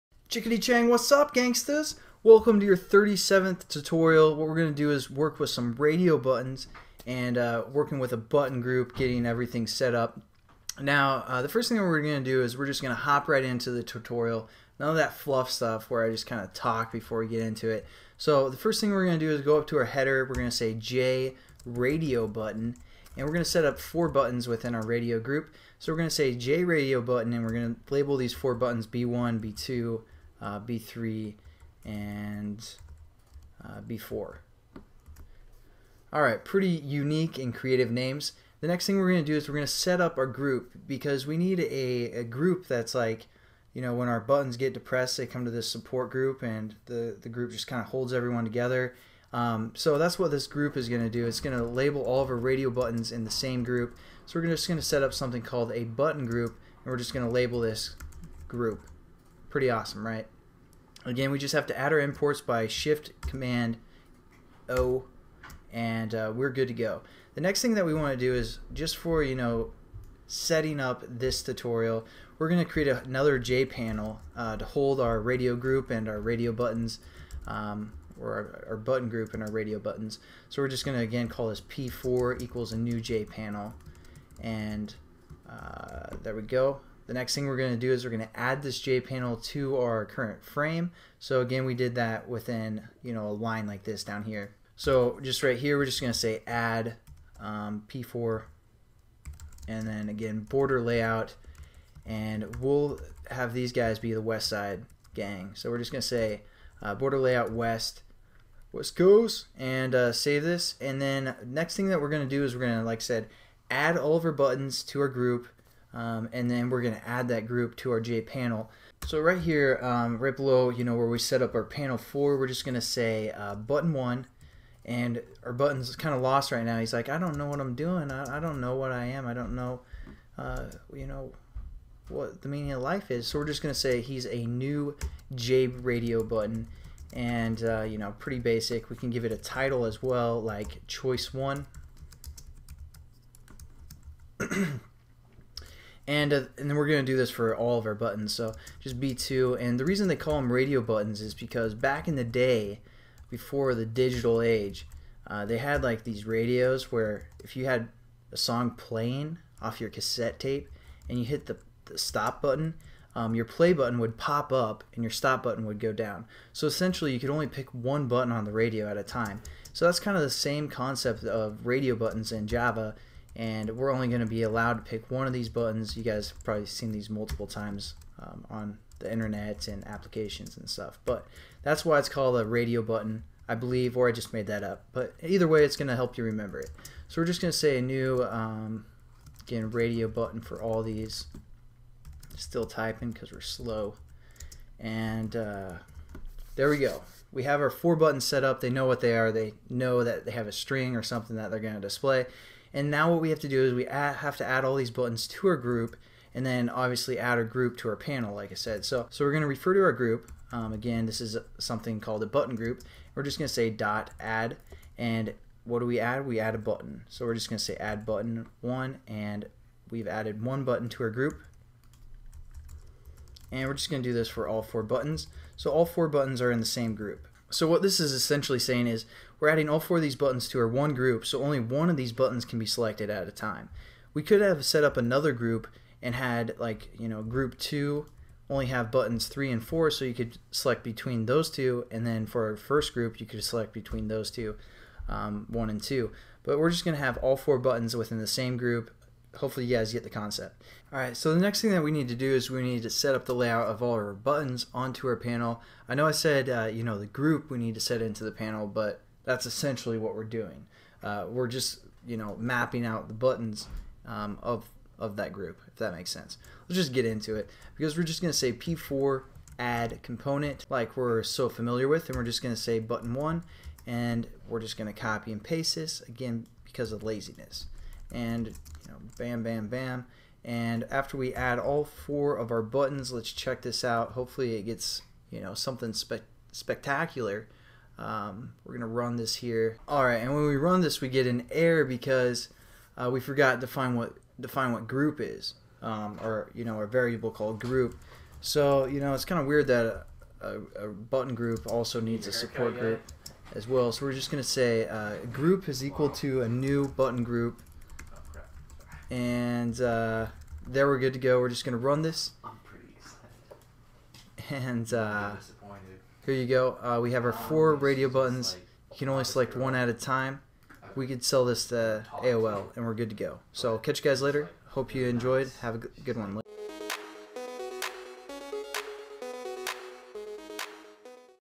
Learn Java Tutorial 1.37- Radio Buttons and Button Group
In this Java tutorial we will learn about radio buttons and button groups.  Radio buttons allow us to check only one button within a certain group.